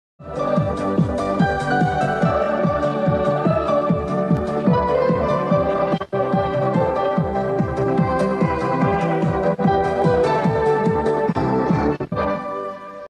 ID Canal 10 Uruguay (2002-2006)
ID usando entre amediados de 2002 hasta 2006

Créditos a @joseantoniolavie que tenia el ID en buena calidad